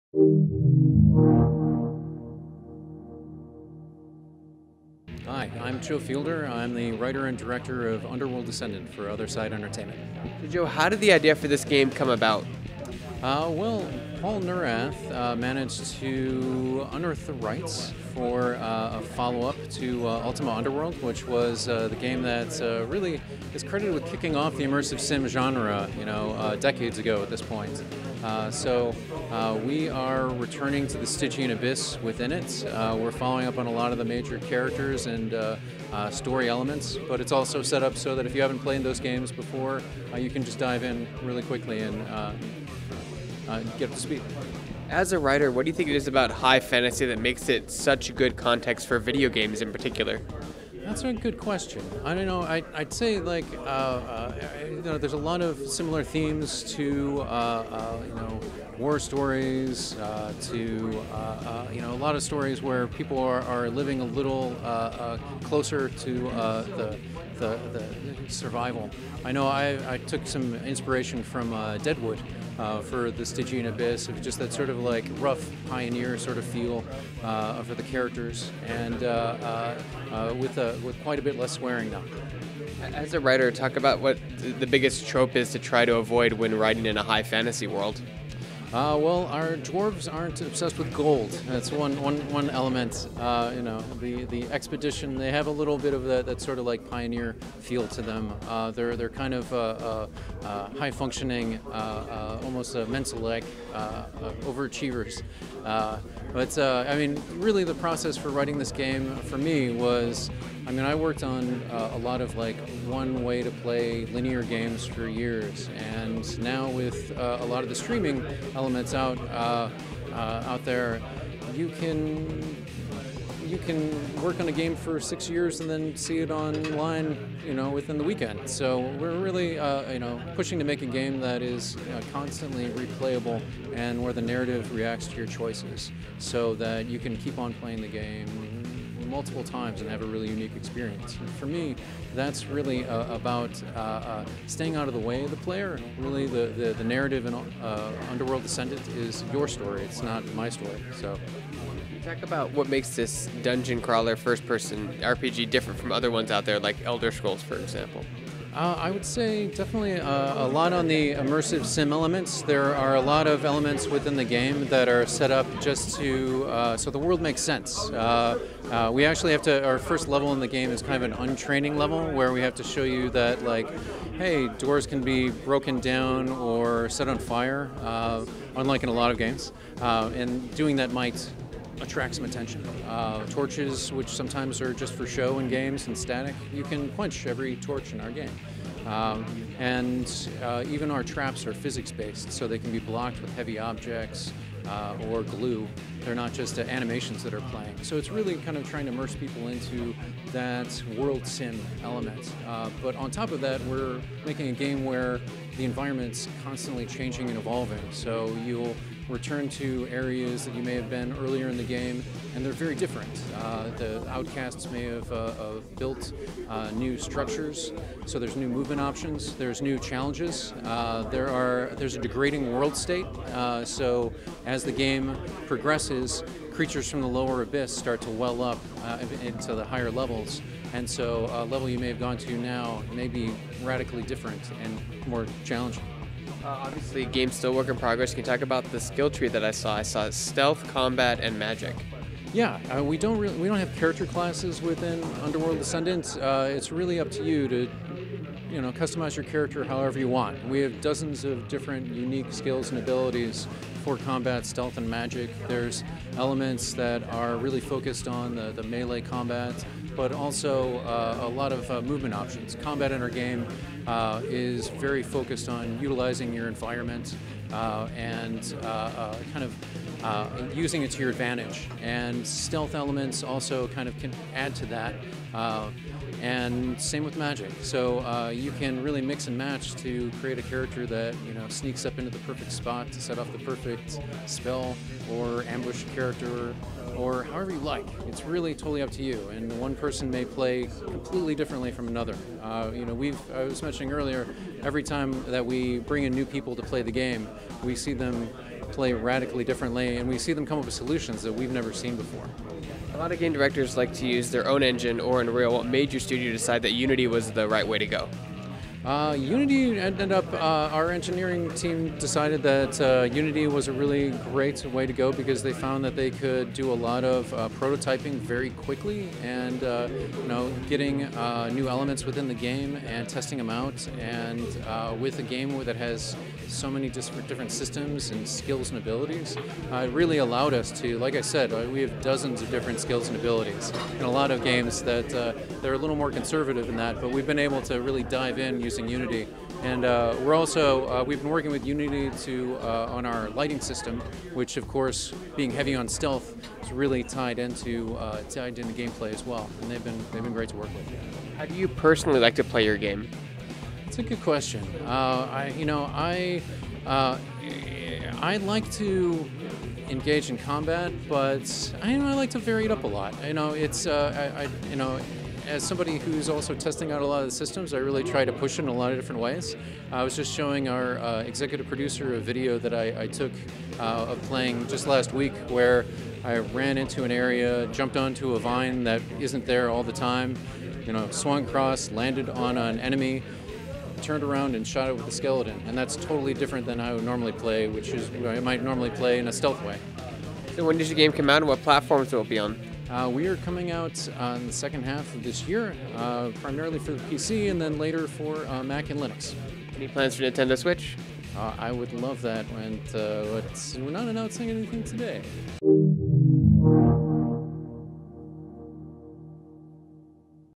Underworld Ascendant - Developer Interview
Underworld Ascendant is an upcoming first-person action role-playing game developed by Otherside Entertainment and published by 505 Games. It is the sequel to Ultima Underworld: The Stygian Abyss and Ultima Underworld II: Labyrinth of Worlds.